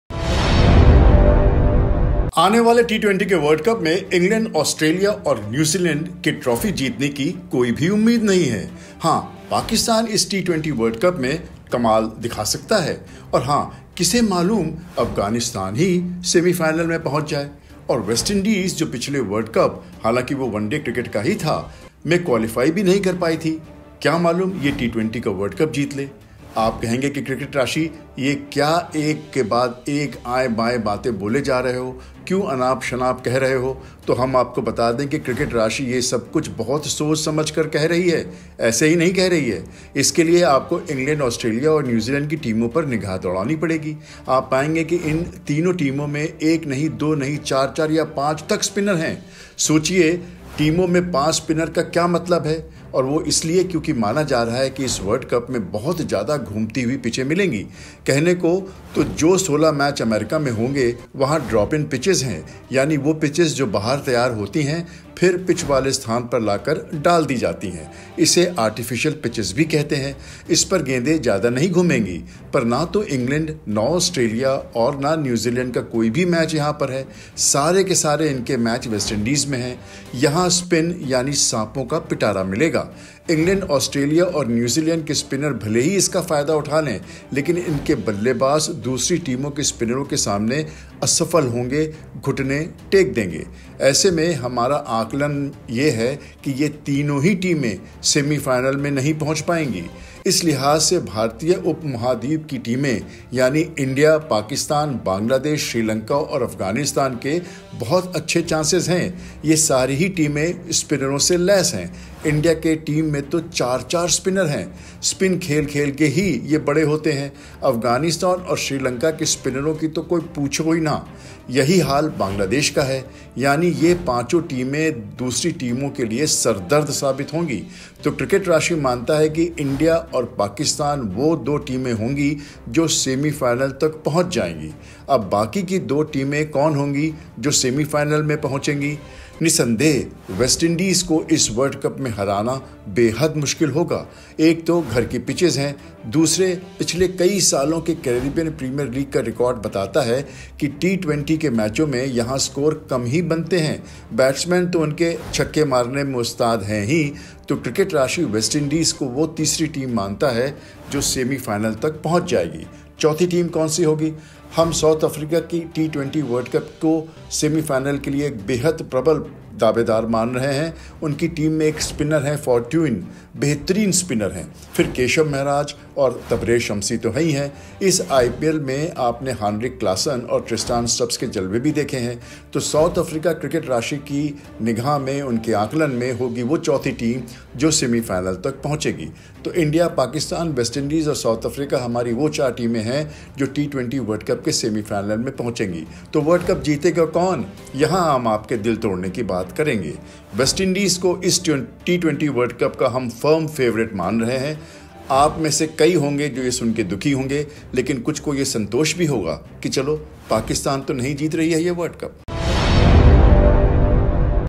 T20 World Cup में ये टीम होगी Rohit Sharma की नासूर!
T20 World Cup में ये टीम होगी Rohit Sharma की नासूर! T20 World Cup: This team will be nightmare for Rohit Sharma’s men!

संयुक्त राज्य अमेरिका और वेस्ट इंडीज में आयोजित होने वाले आगामी टी20 विश्व कप के चार सेमीफाइनलिस्ट कौन होंगे? क्रिकेटराशि का मानना ​​है कि भारत के पास सेमीफाइनल में पहुंचने की पूरी संभावना है। लेकिन उसे संदेह है कि वह विश्व कप जीत पाएगा। तो हम फर्म के पसंदीदा के रूप में किसे रेटिंग देते हैं?

Who would be the four semi-finalists of the upcoming T20 World Cup, to be staged in United States and the West Indies? CricketRashi believes India has all the chances to make it to the semi-finals. But it doubts it would win the World Cup. So who do we rate as the firm favourites?

Join our Official Channels for best Match Prediction, Analysis, Best Fantasy Cricket Teams, Free Giveaways, Cricket News and Updates etc...